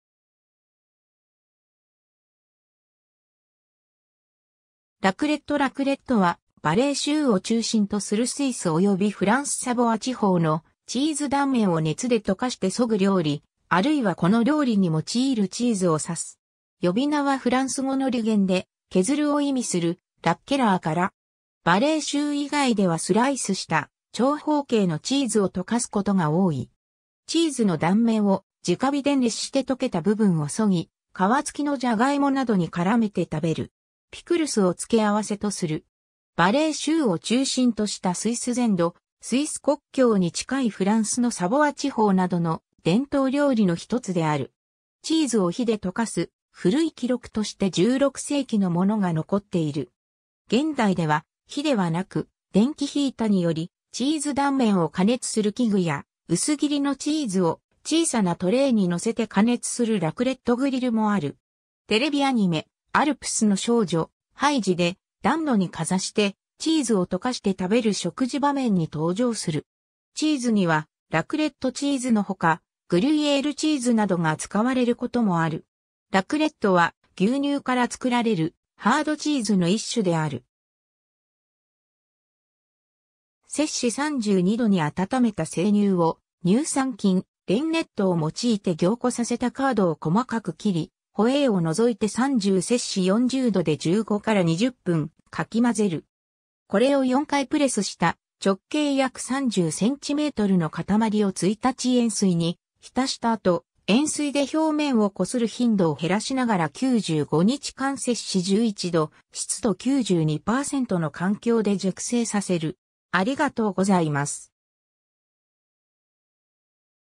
ラクレット
ラクレット（スイス ヴァレー州） ラクレット（フランス語:raclette）は、ヴァレー州を中心とするスイスおよびフランス サヴォア地方のチーズ断面を熱で溶かして削ぐ料理、あるいはこの料理に用いるチーズを指す。
呼び名はフランス語の俚言（パトワ）で「削る」を意味する"racler"から。
ヴァレー州以外ではスライスした長方形のチーズを溶かすことが多い。
 チーズの断面を直火で熱して溶けた部分を削ぎ、皮付きのジャガイモなどに絡めて食べる。
ピクルスを付け合わせとする。
ヴァレー州を中心としたスイス全土、スイス国境に近いフランスのサヴォア地方などの伝統料理の1つである。
チーズを火で溶かす古い記録として16世紀のものが残っている。
現代では火ではなく、電気ヒータによりチーズ断面を加熱する器具や、薄切りのチーズを小さなトレイに載せて加熱するラクレットグリルもある。
 テレビアニメ『アルプスの少女ハイジ』で、暖炉にかざしてチーズを溶かして食べる食事場面に登場する。
 チーズにはラクレットチーズ（後述）の他、 グリュイエールチーズなどが使われることもある。
 ラクレットは、牛乳から作られるハードチーズの一種である。
32℃に温めた生乳を、乳酸菌（RA401）、レンネット（凝乳酵素）を用いて凝固させたカードを細かく切り、ホエーを除いて30～40℃で15～20分かき混ぜる。
これを4回プレスした直径約30 cmの塊を1日塩水に浸した後、塩水で表面をこする頻度を減らしながら95日間11℃、湿度92％の環境で熟成させる。